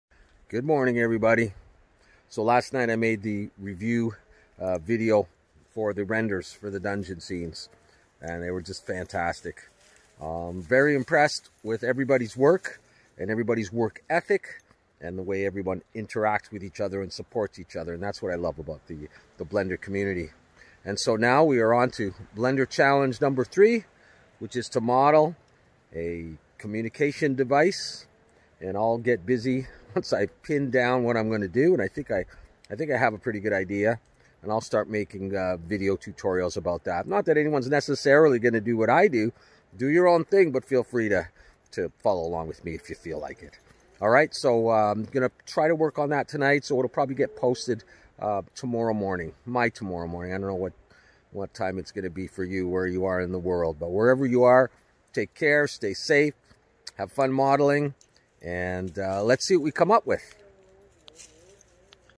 BLENDER CHALLENGE 3: Modeling a Cassette Player Video Series
All of the videos in the series showing how I model a CASSETTE PLAYER for Blender Challenge 3 are linked to below.  You can also find the link to the discord server so you can join in the discussions and post your WIPs and final render for this challenge and future challenges.

BLENDER CHALLENGE 3: COMMUNICATION DEVICE
Deadline: Sunday October 25, 2020

MODELING A CASSETTE PLAYER VIDEO SERIES
Part 8: Render Review ... coming soon